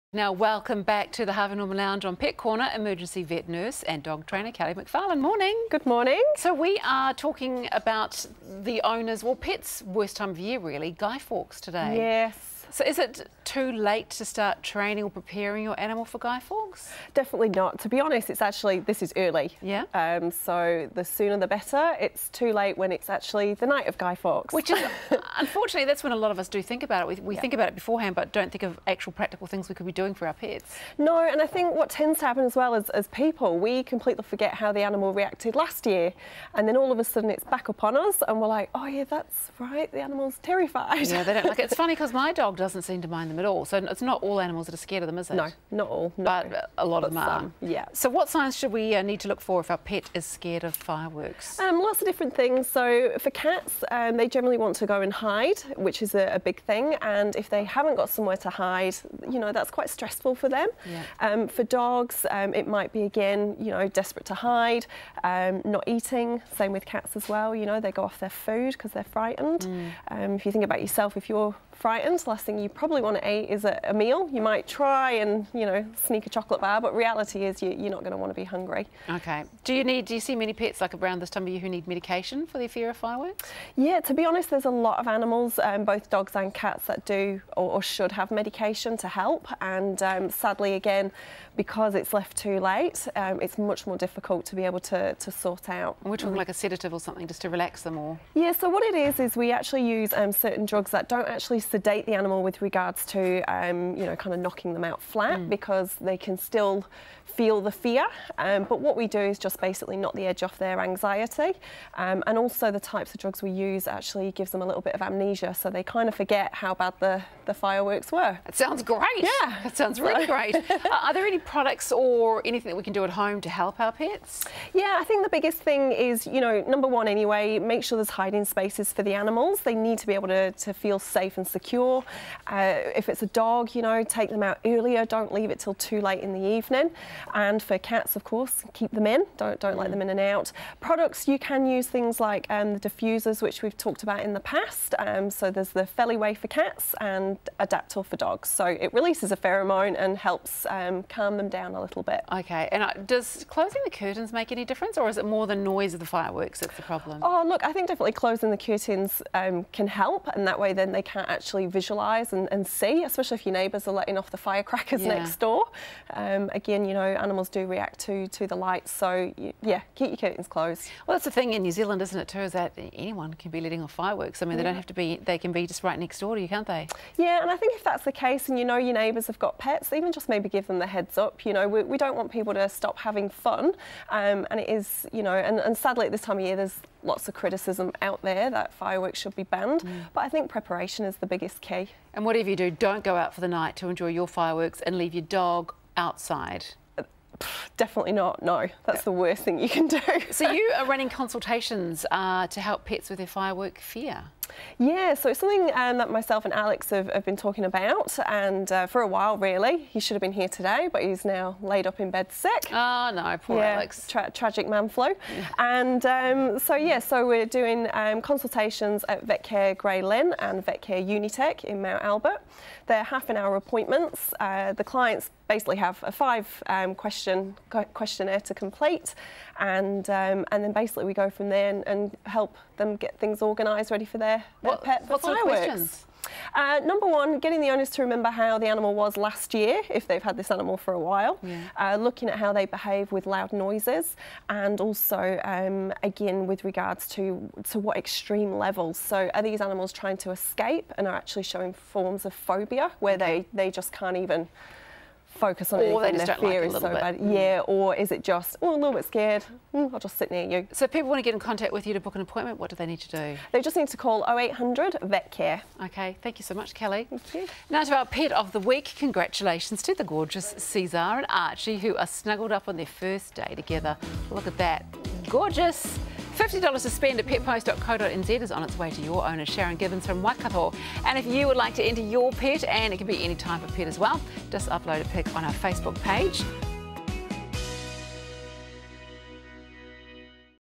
Guy Fawkes and your Pets
This morning on Pet Corner this morning Emergency Vet Nurse and Dog Trainer Kelly McFarlane joins us to talk about getting your pets ready for Guy Fawkes.